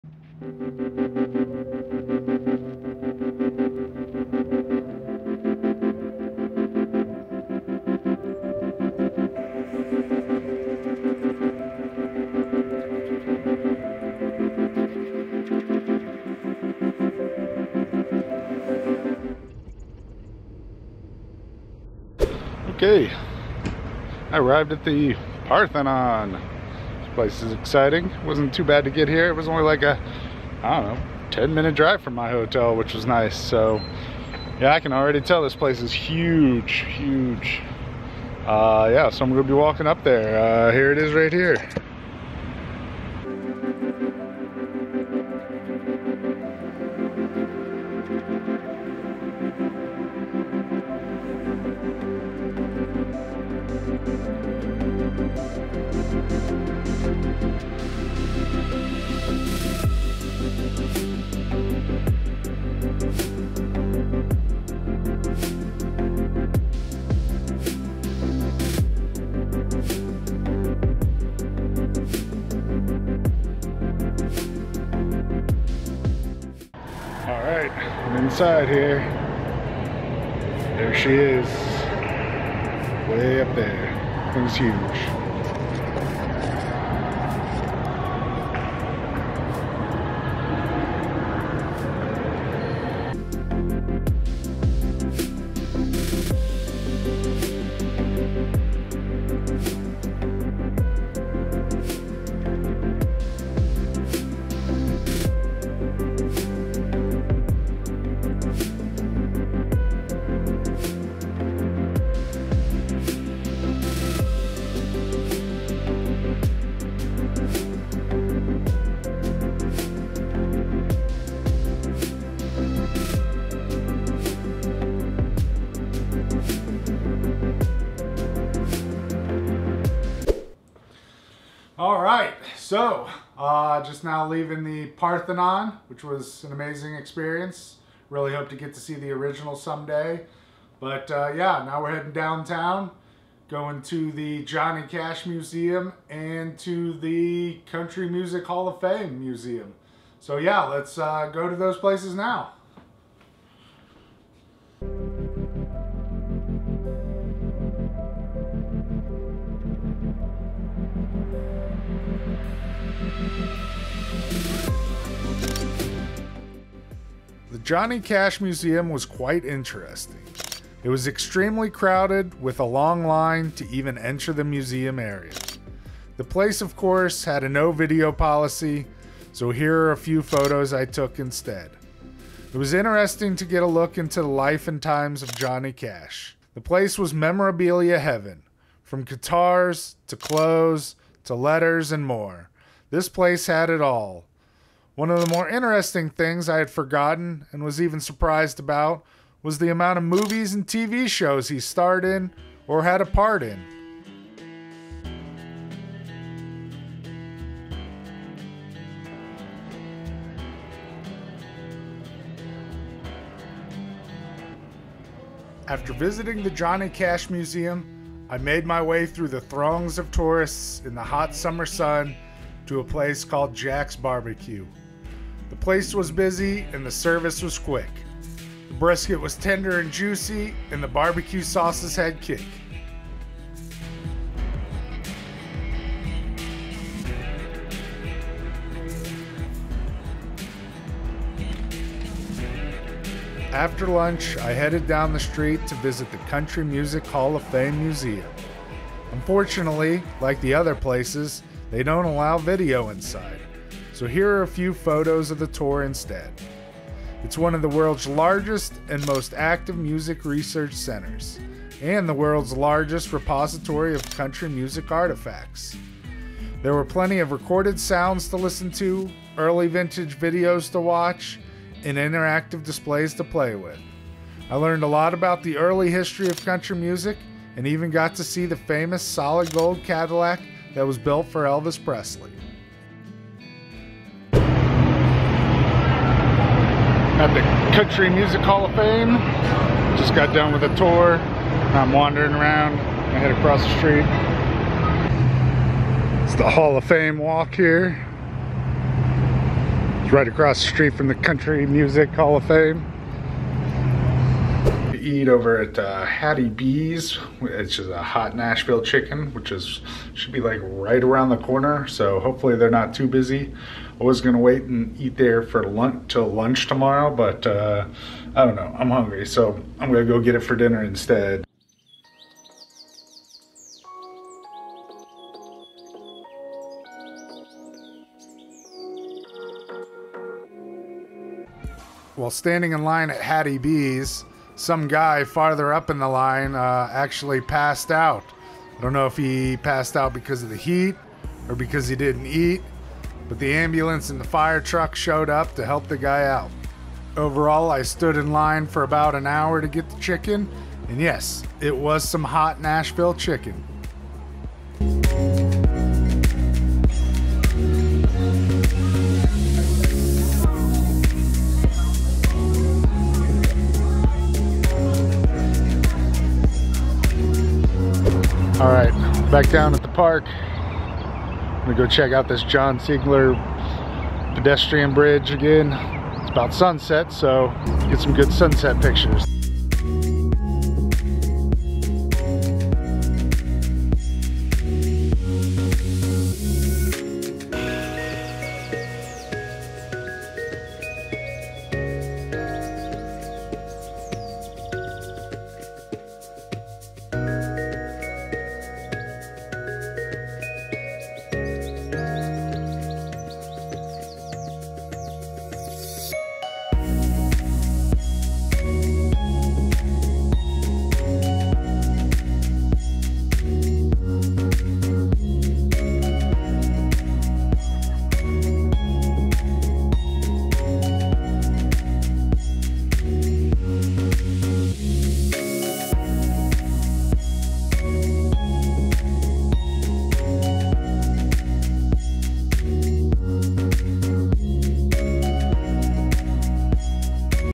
Ambulance Was Called
Day 2 of my trip to Nashville. I started off visiting the Parthenon, then I visit the Johnny Cash museum and hit up the Country music hall of fame museum after lunch. Also ate some Hot Nashville chicken were emergency personnel were called due to a guy passing out in line.